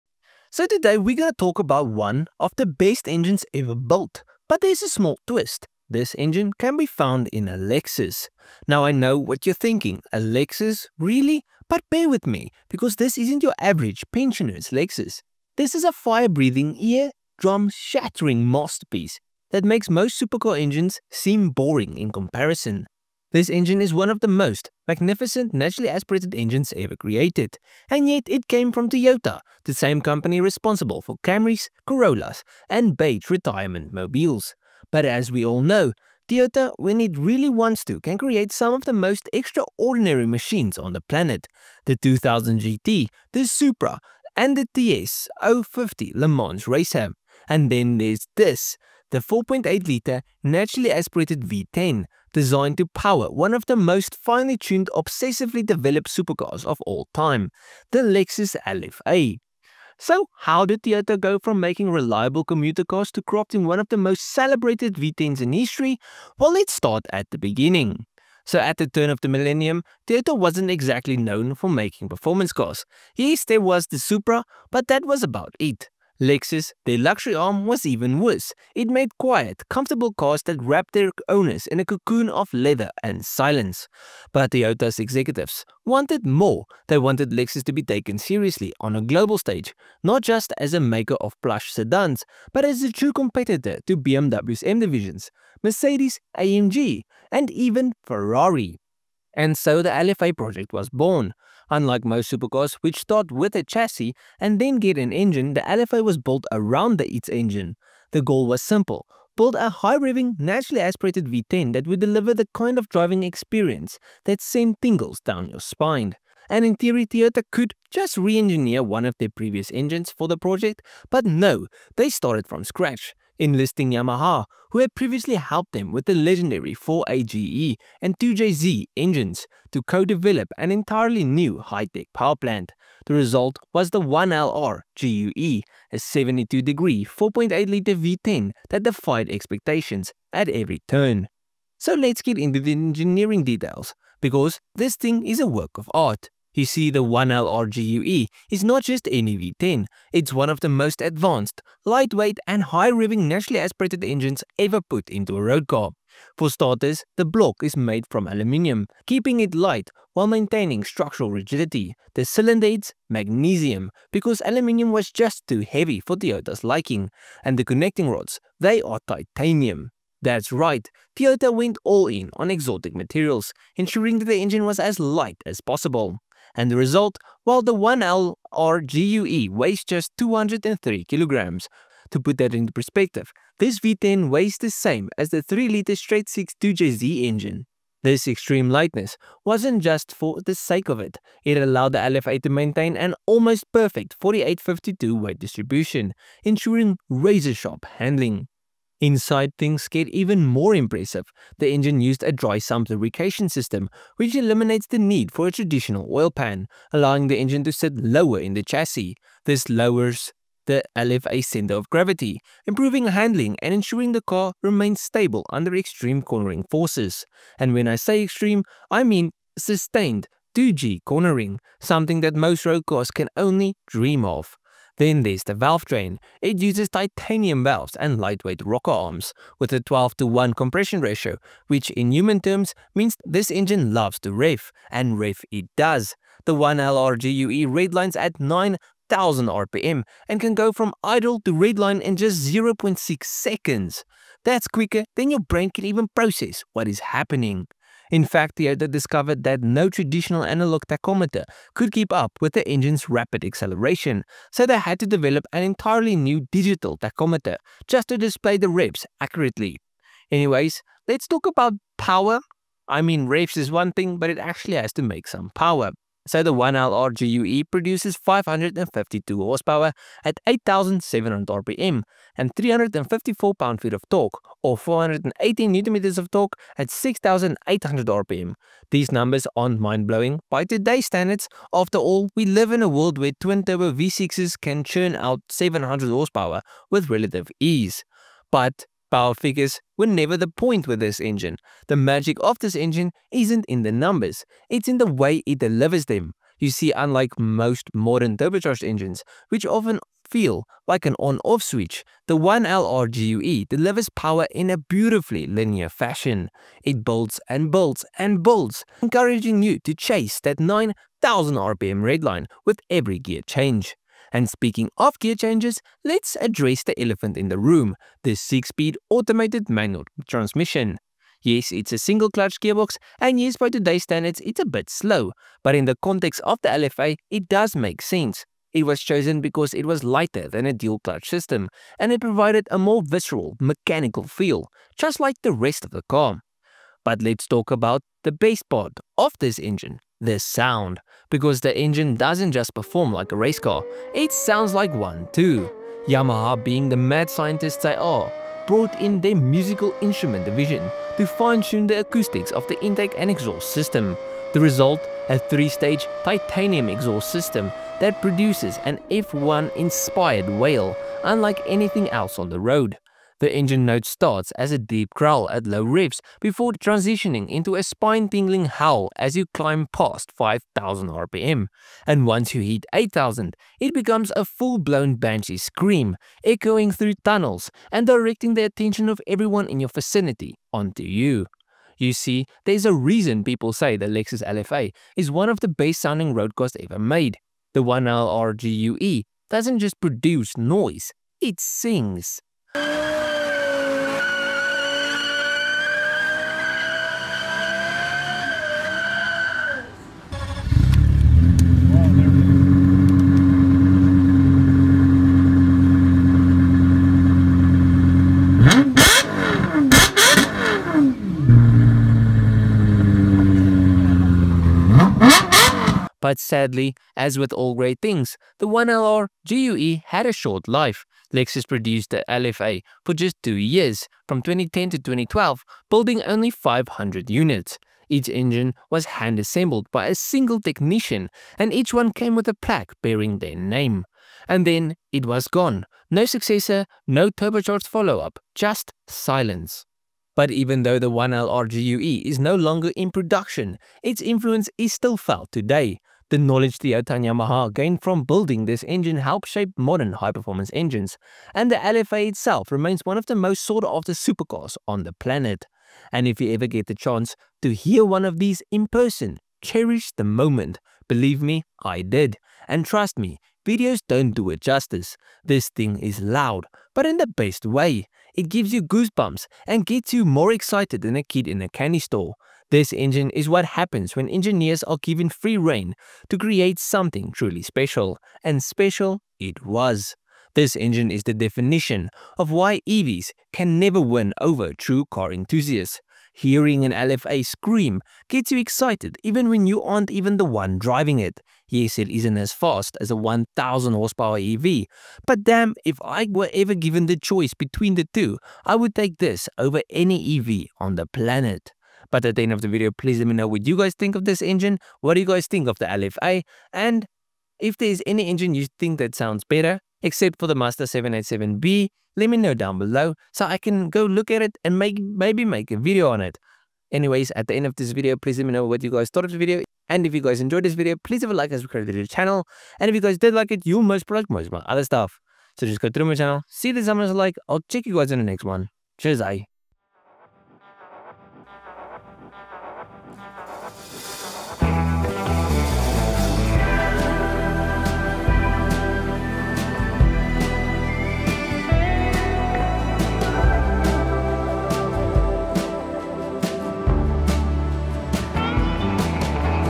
The Lexus 1LR GUE A 9,000 RPM Symphony of Engineering Genius
So today we are going to talk about one of the best engines ever build but there is a small twist this engine can be found in a Lexus. Now, I know what you're thinking: "A Lexus? Really?" But bear with me, because this isn't your average pensoners lexus. This is a fire-breathing, eardrum-shattering masterpiece that makes most supercar engines seem boring in comparison. This engine is one of the most magnificent naturally aspirated engines ever created, And yet, it came from Toyota—the same company responsible for Camrys, Corollas, and beige retirement-mobiles. But as we all know: Toyota, when it really wants to, can create some of the most extraordinary machines on the planet. The 2000GT, the Supra, the TS050 Le Mans racer. And then there’s this—a 4.8-liter, naturally aspirated V10, designed to power one of the most finely tuned, obsessively developed supercars of all time: the Lexus LFA.